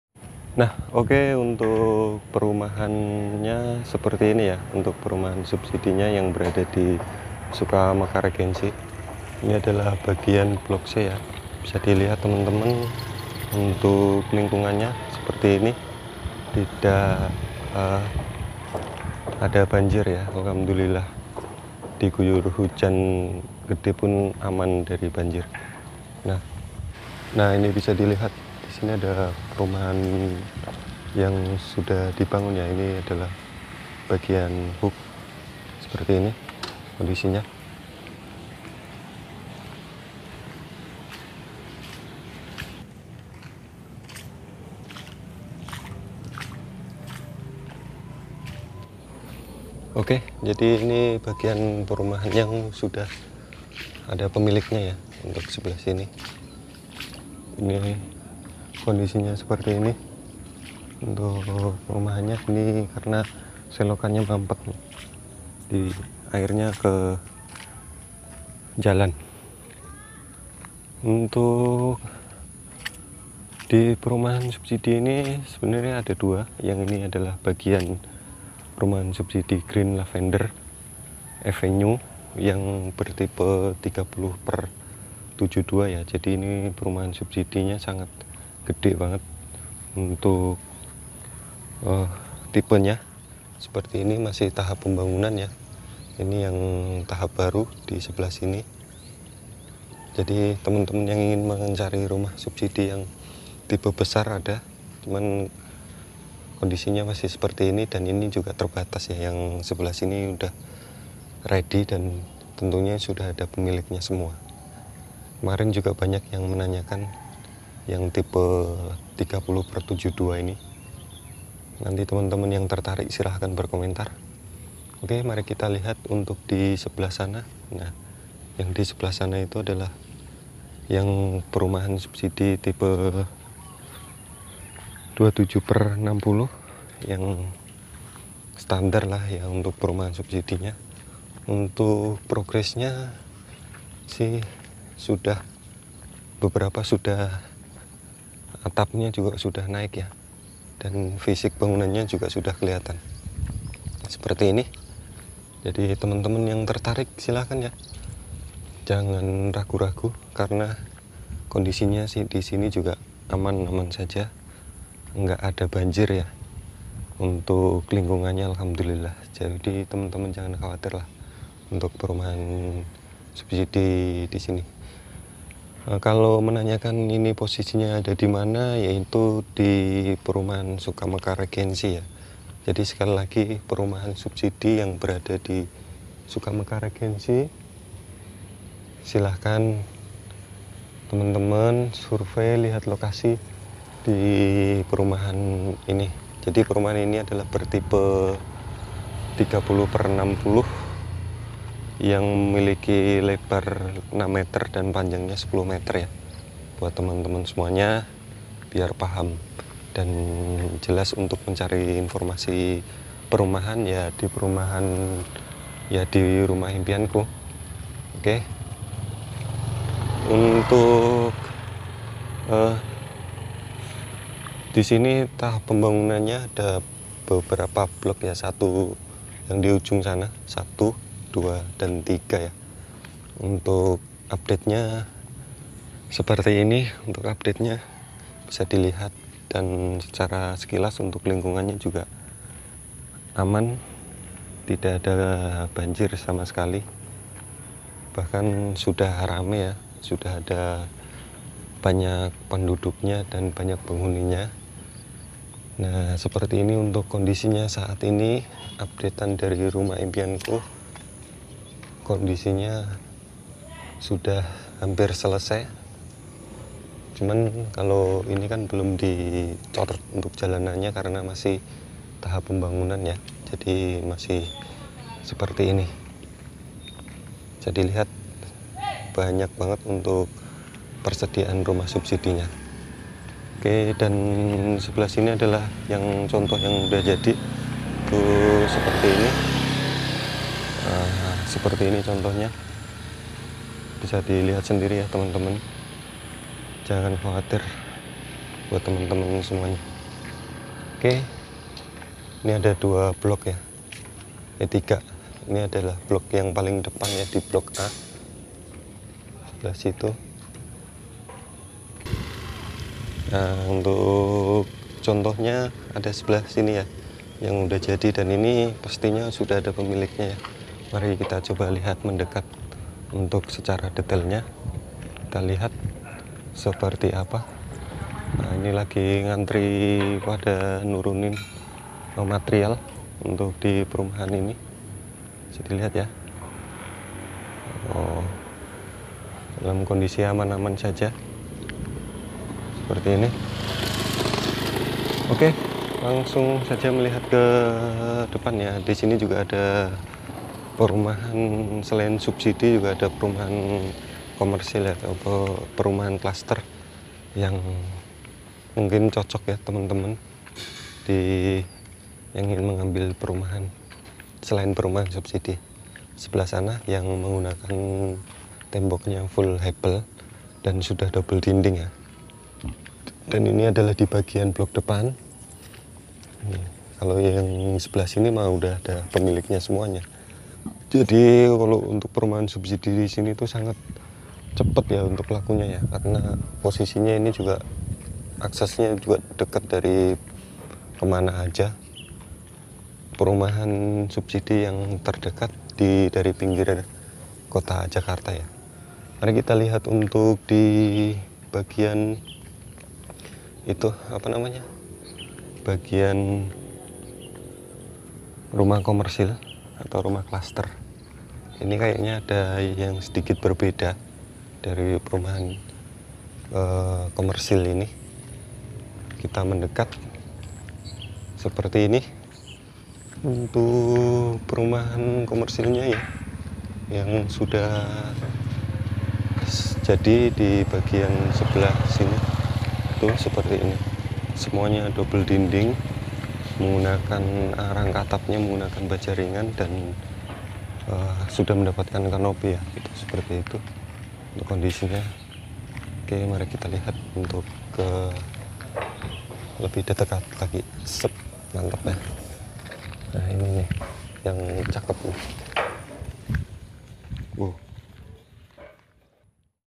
alhamdulillah tidak banjir meski di guyur hujan lebat-rumah subsidi terbaru 2023
tebukti perumahan subsidi di sini tidak banjir meski sering di guyur hujan lebat setiap hari.
ini adalah video yang sering di tanyakan teman teman kepada rumah impianku.
seperti apa kondisi perumahanya silahkan simak video sampe abis ya.
trimakasih

buat teman teman yang sudah selalu suport channel rumah impianku dengan cara...

SEMOGA BERMANFAAT 🙏